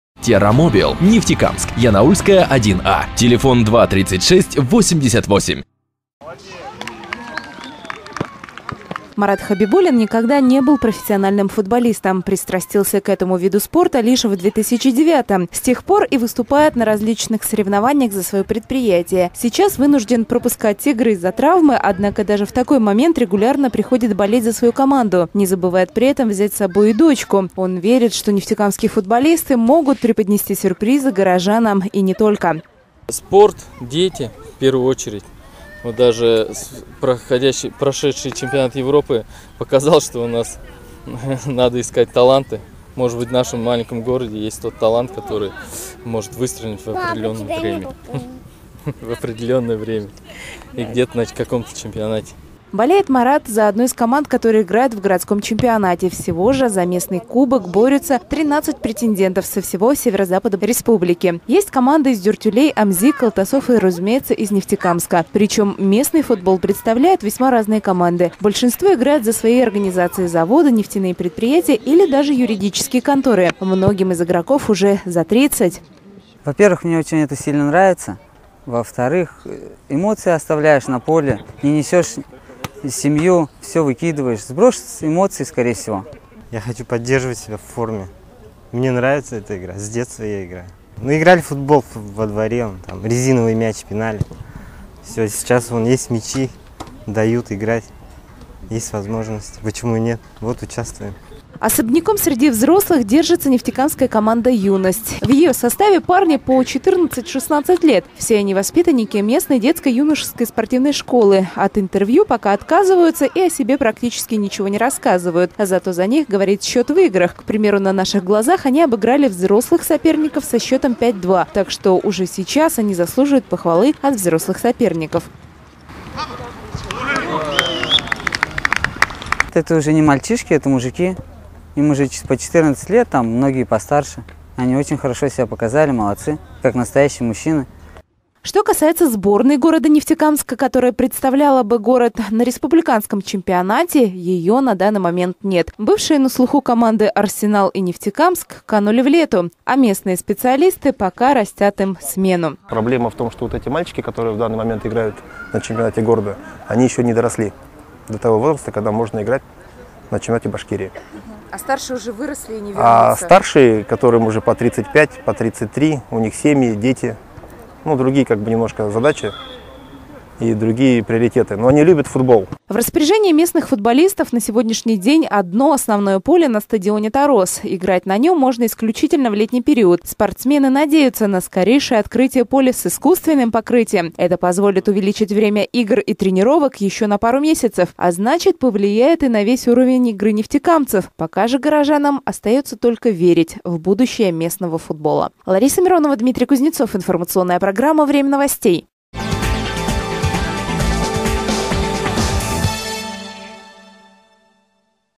Футбол в Нефтекамске: скорее жив, чем мертв?
Прошедший чемпионат Европы по футболу принес российским болельщикам и радость и огорчение. Последние чувство возникло в основном из-за неубедительной игры наших соотечественников. После чего большинство болельщиков стали говорить о необходимости поднятия футбола на местах и поиска талантов в провинции. Как обстоят дела с футболом в Нефтекамске, и можно ли здесь найти будущих «королей мяча» расскажет Лариса Миронова.